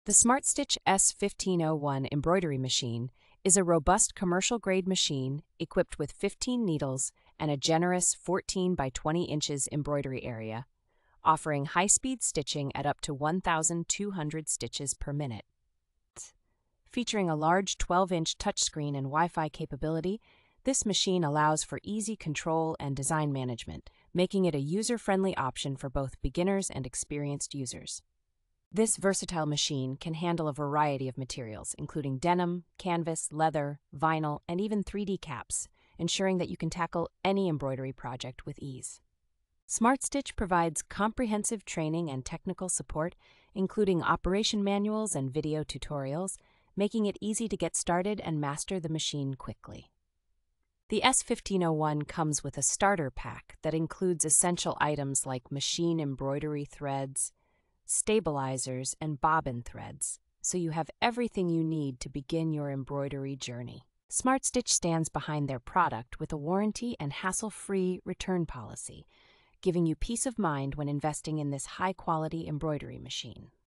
REVIEW (2025): Smartstitch S-1501 Embroidery Machine. ESSENTIAL details.
The Smartstitch S-1501 Embroidery Machine is a robust commercial-grade machine equipped with fifteen needles and a generous fourteen by twenty inches embroidery area, offering high-speed stitching at up to one thousand two hundred stitches per minute.

Featuring a large twelve-inch touch screen and WiFi capability, this machine allows for easy control and design management, making it a user-friendly option for both beginners and experienced users.

This versatile machine can handle a variety of materials, including denim, canvas, leather, vinyl, and even 3D caps, ensuring that you can tackle any embroidery project with ease.

Smartstitch provides comprehensive training and technical support, including operation manuals and video tutorials, making it easy to get started and master the machine quickly.

The S-1501 comes with a starter pack that includes essential items like machine embroidery threads, stabilizers, and bobbin threads, so you have everything you need to begin your embroidery journey.

Smartstitch stands behind their product with a warranty and hassle-free return policy, giving you peace of mind when investing in this high-quality embroidery machine.